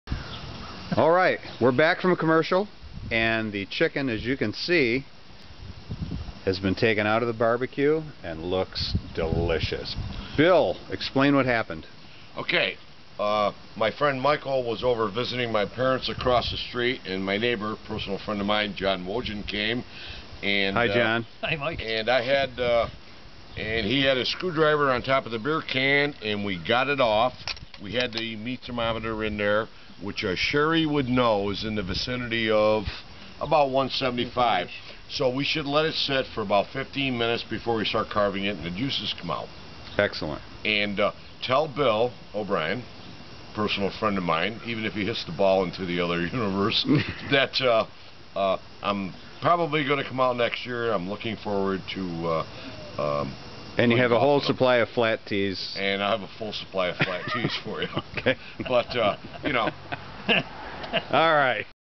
2007 - Beer Can Chicken - Chapter 2
Bumper Gressler and John Wojden of Little Falls, NY explaining how to cook a "Beer Can Chicken" on the gas BBQ.  Chapter 2 of 2.